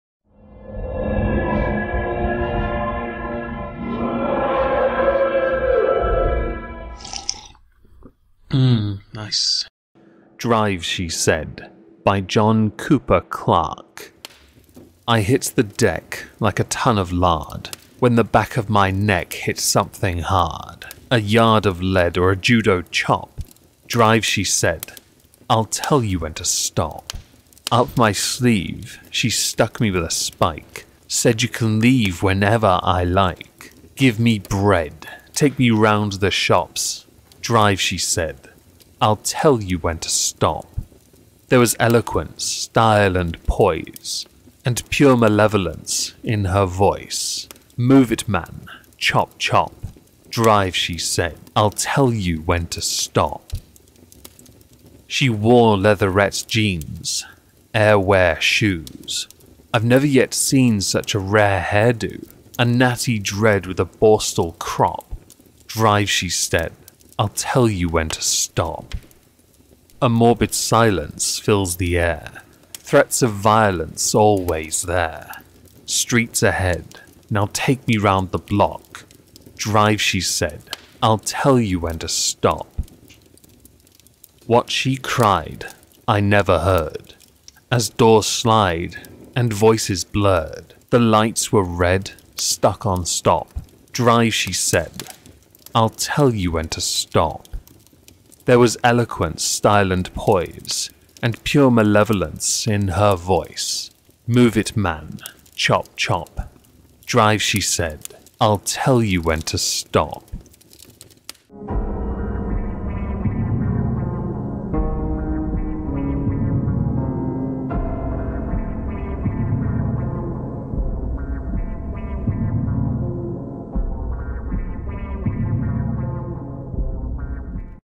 Drive She Said by John Cooper Clarke
Welcome back to Moistski Reads! Today, we shall be continuing our readings, from the works, of John Cooper Clarke.